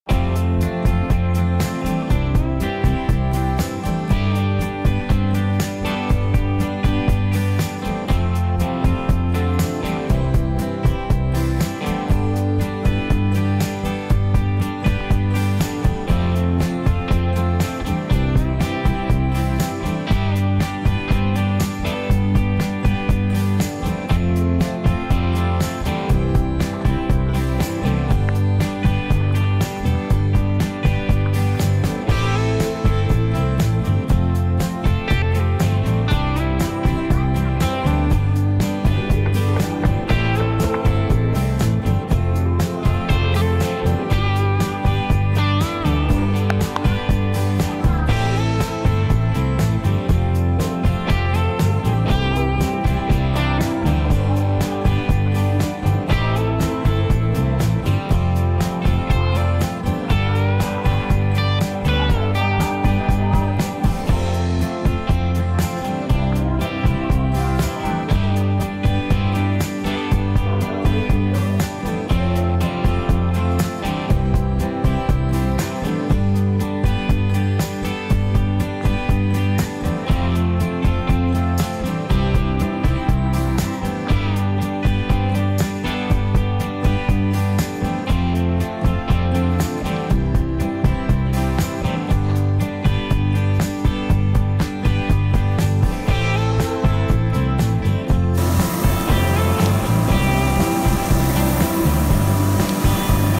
A Day in A life of a Coffee Farmer 😊☕️🍒🇵🇭
Liberica Variety . Balete Batangas